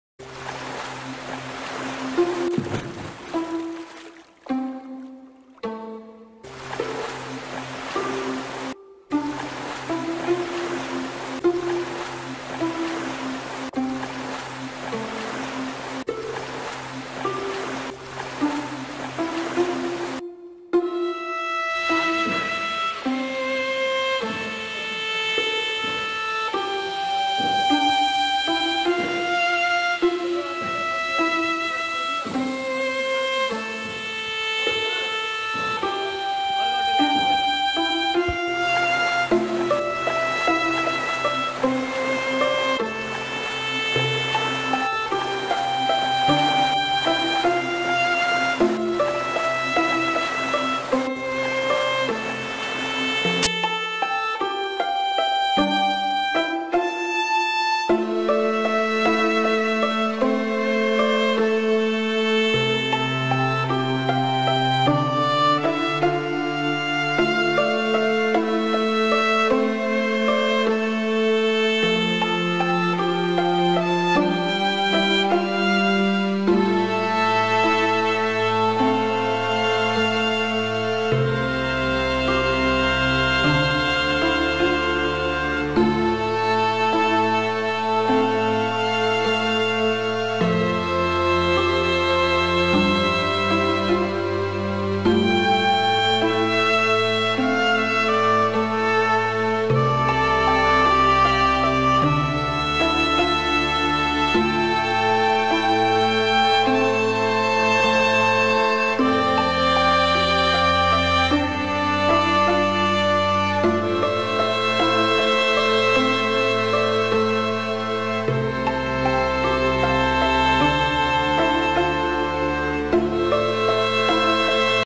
Amazing Waterfall deep inside the jungle
See the enormous jungle with falls and ponds